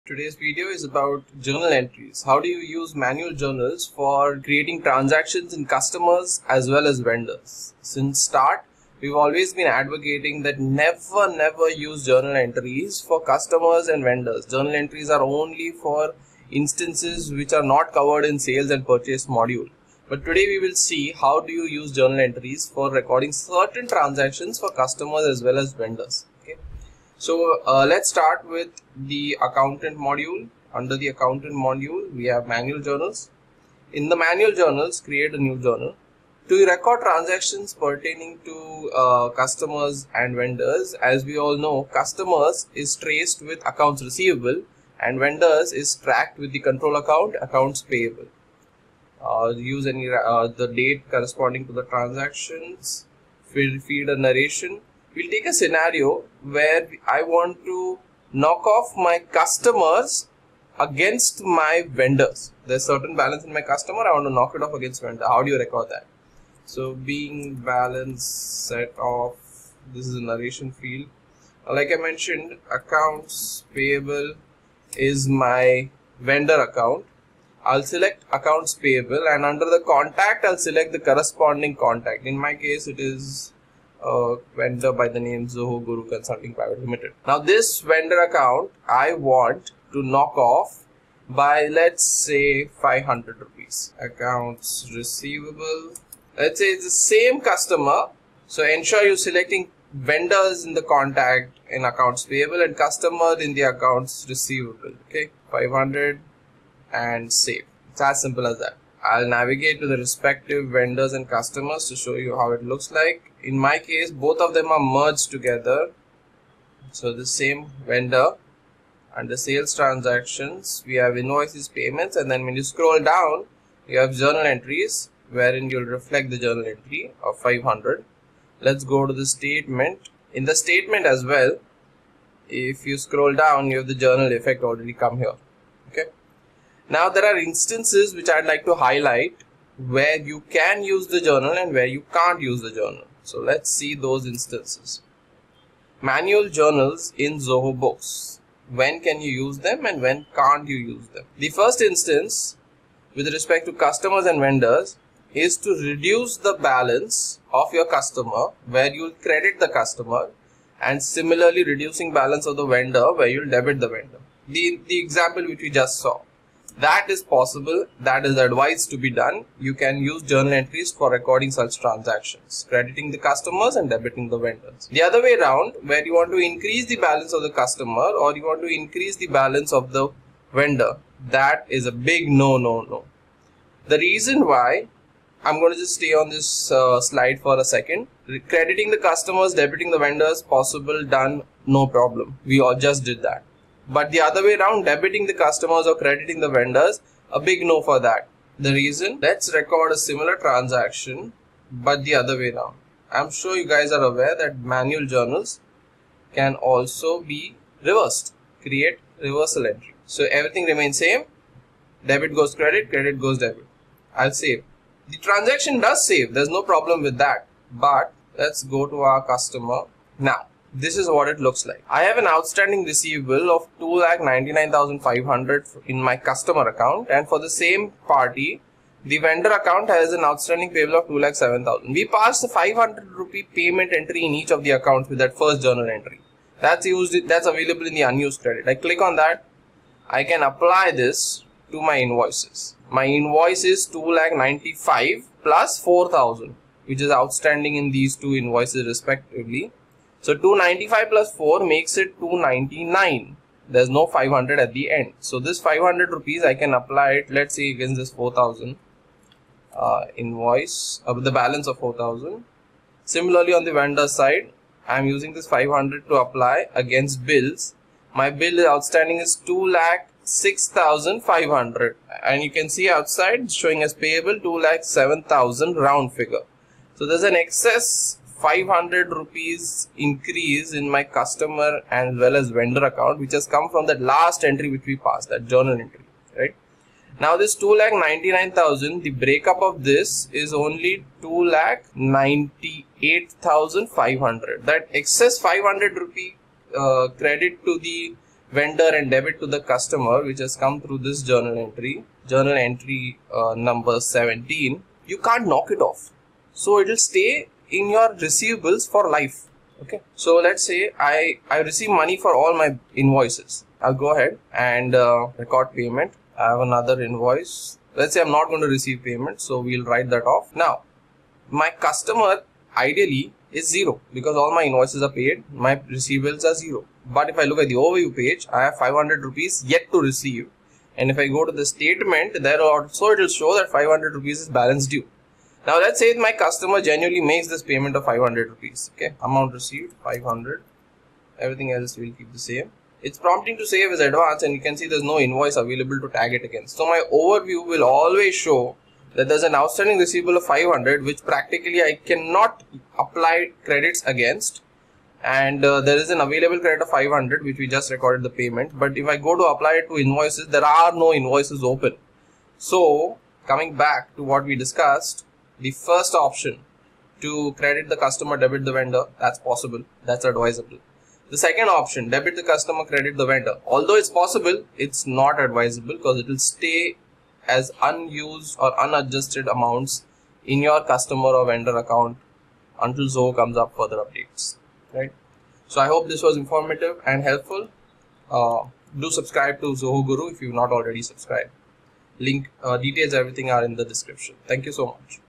Important Update in Zoho Books - JV in Customers and Vendors (In English)
n this video, we dive into the newly introduced JV (Journal Voucher) feature in the Customer and Vendor modules of Zoho Books! 🎉 This has been one of the most anticipated updates by users, and we’re excited to show you how it works.

We’ll cover:

✅ What the JV feature is and its significance
✅ How to create and manage JVs in both Customer & Vendor modules
✅ Practical use cases to help you understand the benefits
✅ Tips for efficient accounting using JVs in Zoho Books

This update is a game-changer for users seeking seamless accounting and streamlined processes.